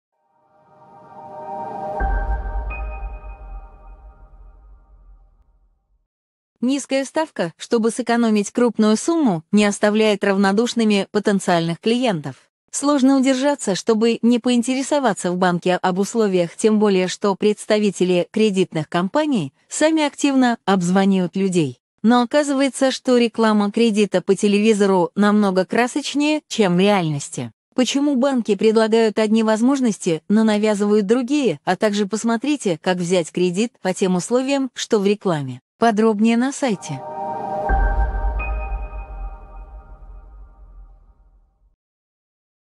Зачем банк рекламирует кредит под низкий процент и не выдает
Почему банки предлагают одни возможности, а навязывают другие, как взять кредит по тем условиям, что в рекламе. Проверим на примере ВТБ, Почта Банка и Сбербанка, как отличается реальная ставка от фактической. Подробнее